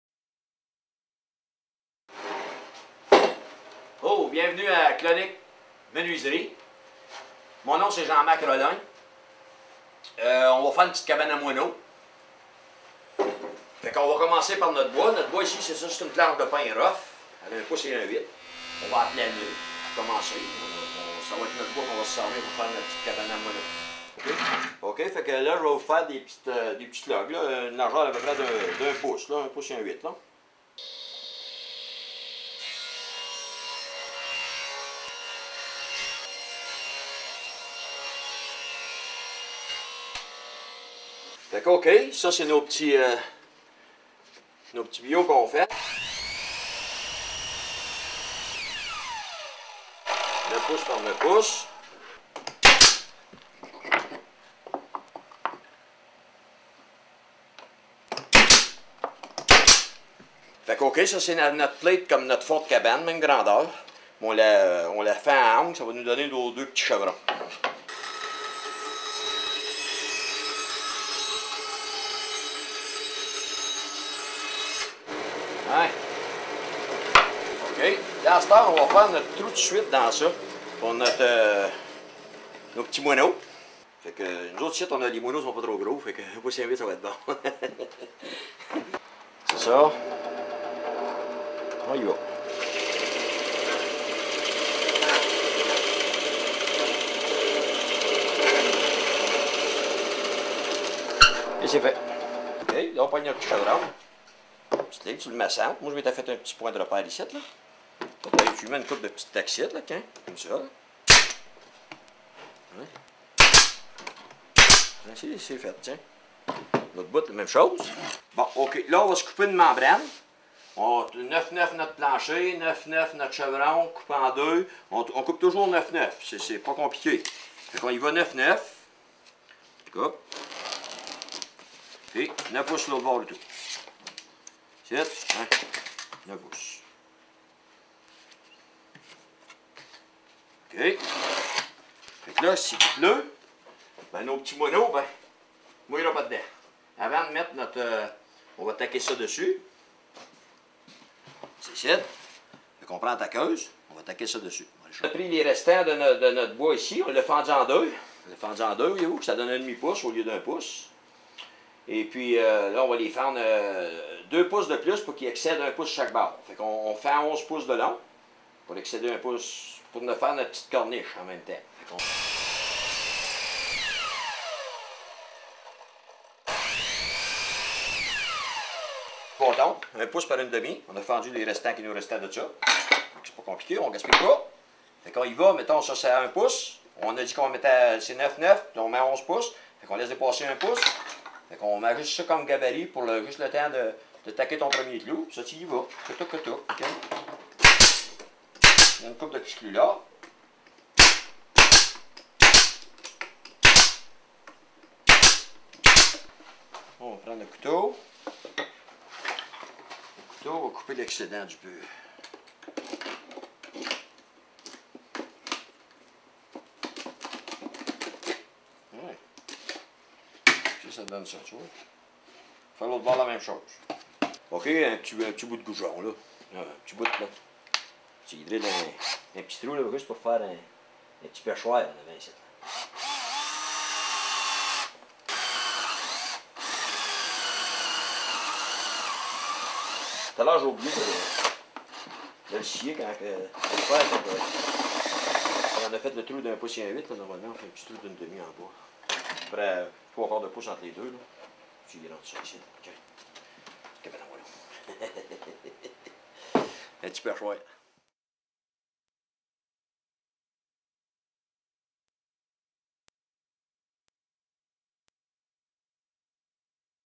Fabriquer une cabane à oiseaux
Comment fabriquer une cabane à oiseaux avec Jean-Marc Rollin.

Réalisé par l'équipe MONOLYTHE. Projet: Neutral HV.

Simon Bélanger, Christian Bergeron , Patrick Lemay , Samuel Moreau , Micheline Rochon, Bianca Ross-Aubin